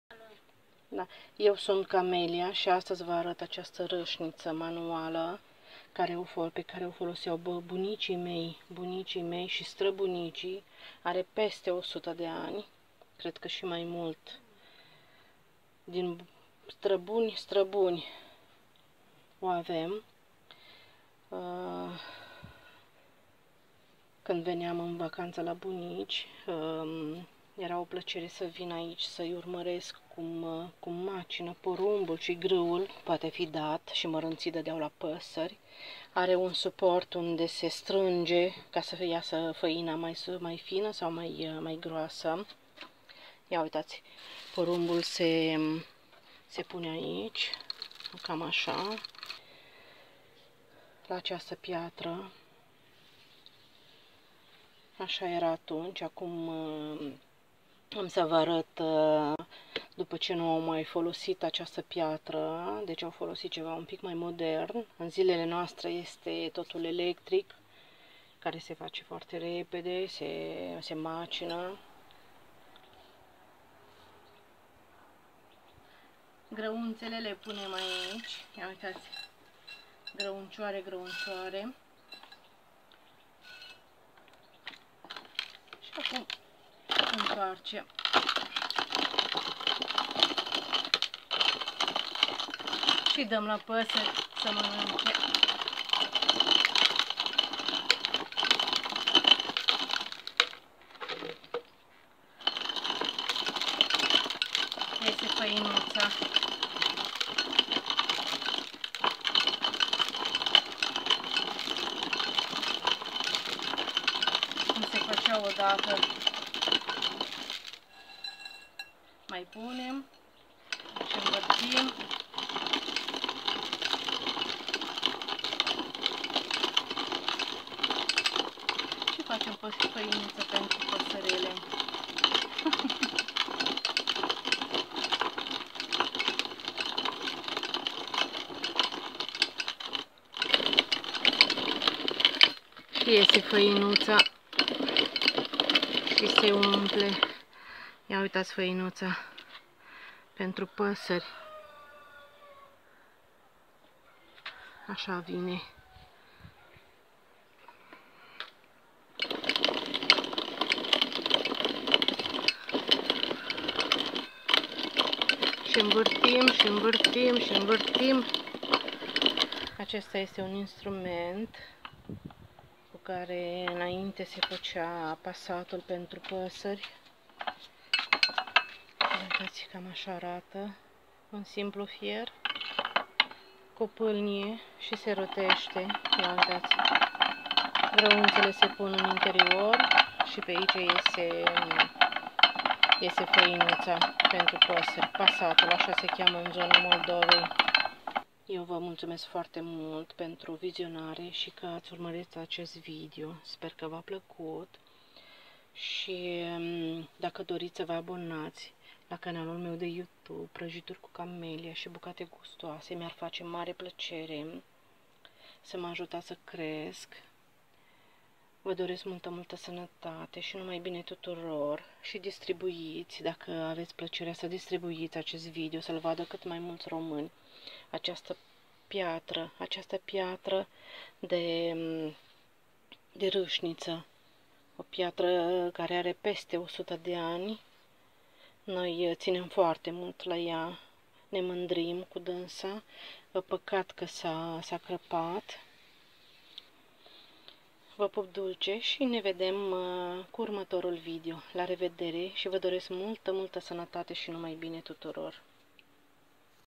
Traditii din strabuni - Cum se macina odata - Macinatul / Rasnitul Porumbului pentru pasari
Acasa la bunici
Dacă ți-a plăcut îi poți da share
Va multumesc!